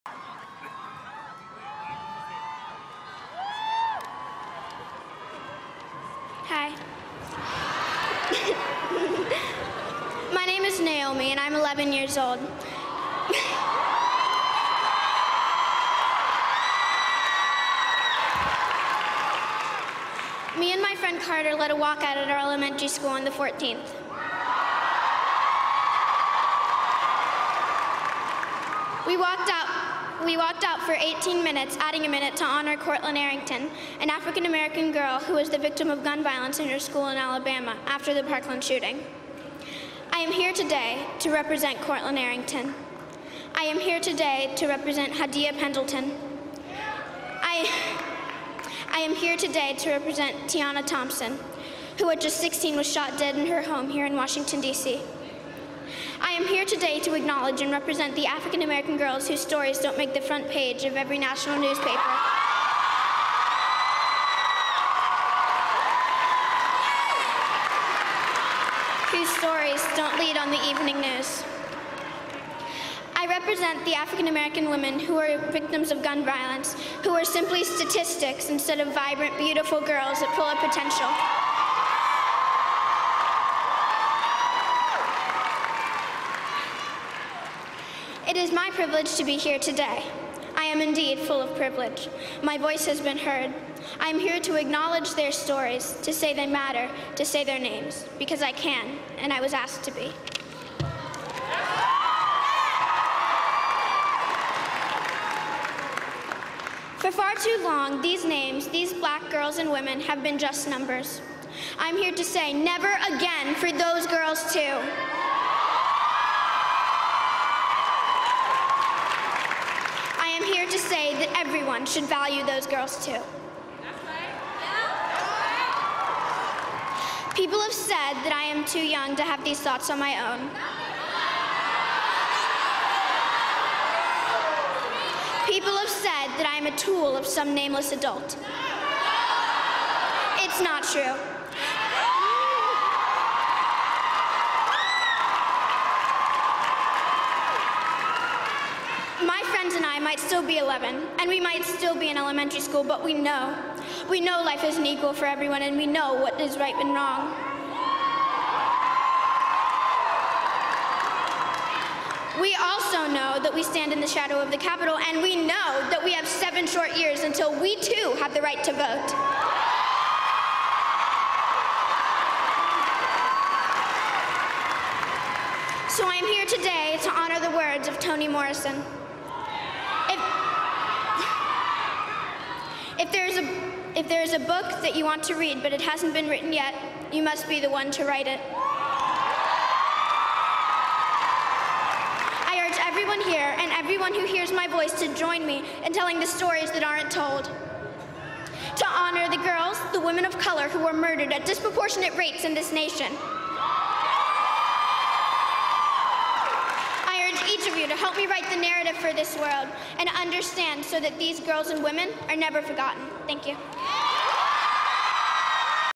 'I Am Here To Say Never Again': 11-Year-Old At March For Our Lives
Her name is Naomi Walder. She is 11-years-old, a student and an activist. She led a walkout at her elementary school and shared her message at the March For Our Lives